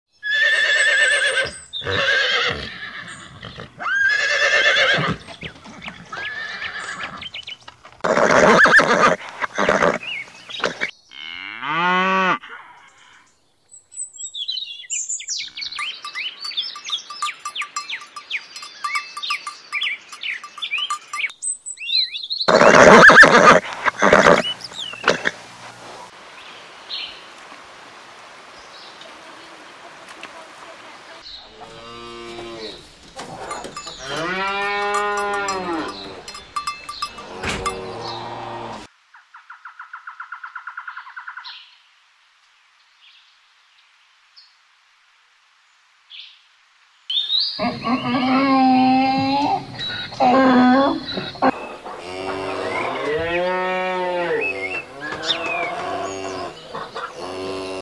Animals world part 45 Horse, cow, donkey
Animal sounds help with irritability and nervousness.
Voices in the video: Horse, cow, donkey